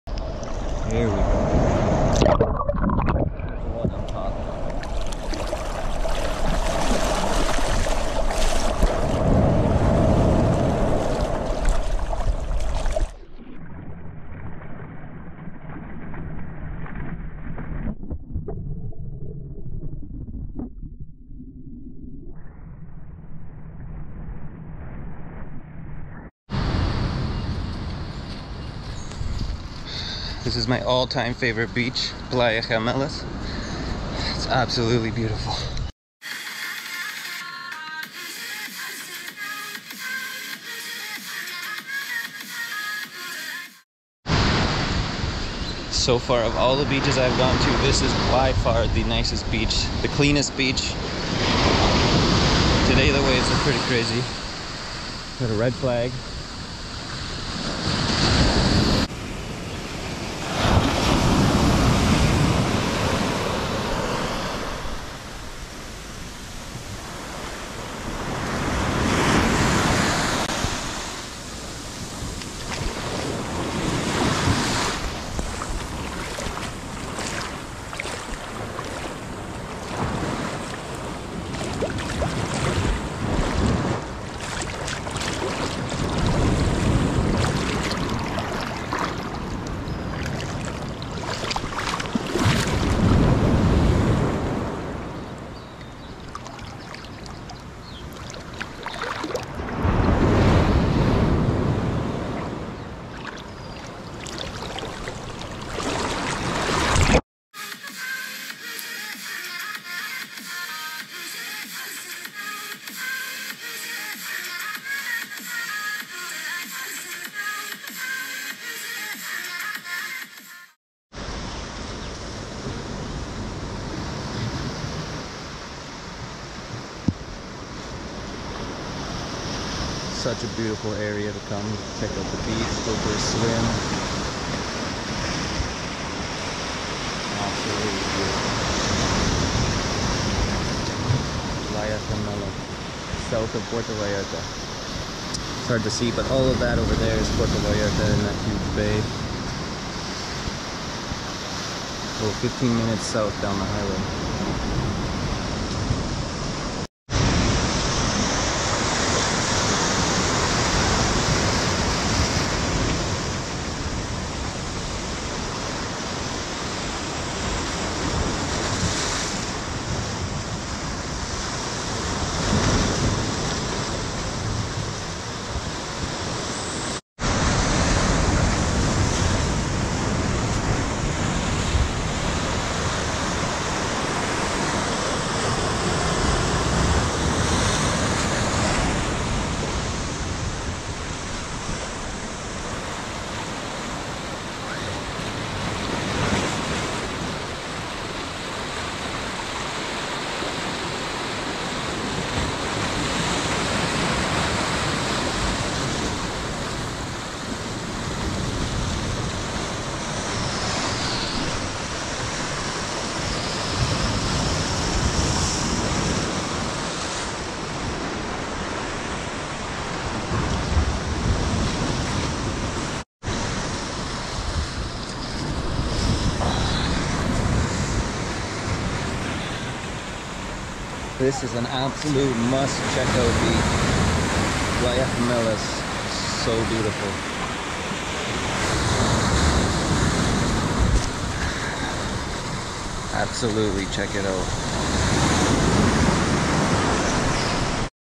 One of the BEST beaches around! | Playa Gemelas | Mexico
Perfect little beach south of Puerto Vallarta about 15 minutes. Once you see the signs for playa Gemelas access find a place to park and follow the stair set down!